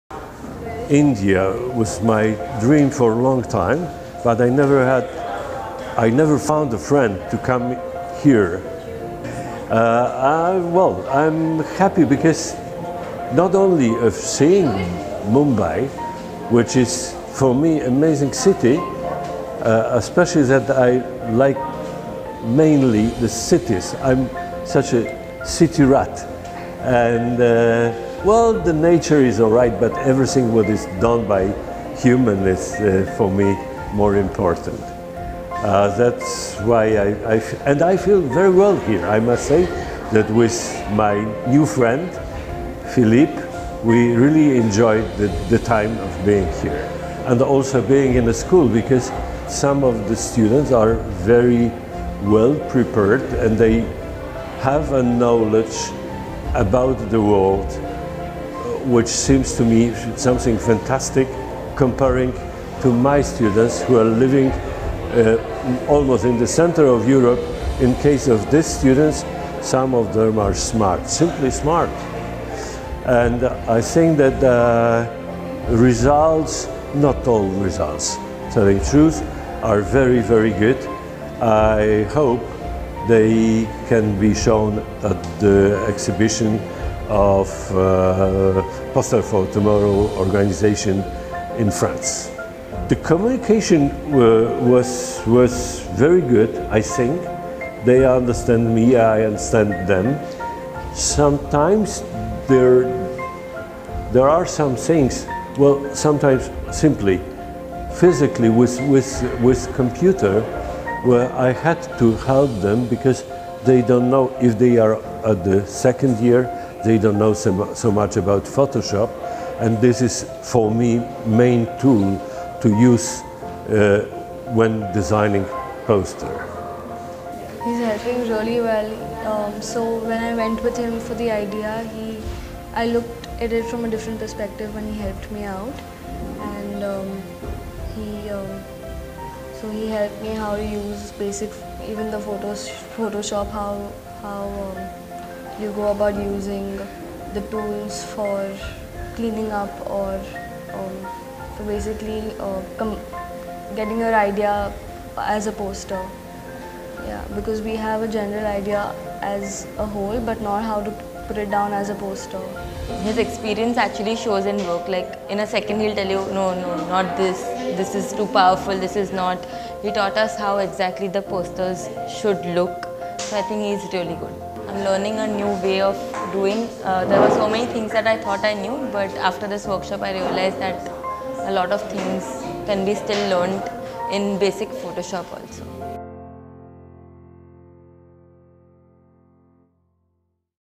International Workshop Piotr Kunce - Poland meets India
We take you on one of the journeys of our International Workshop by Piotr Kunce  - Professor at Academy of Fine Arts - Krakow, Poland. He spent a week with our students on the poster project. Hear him and the students summarize their experience.!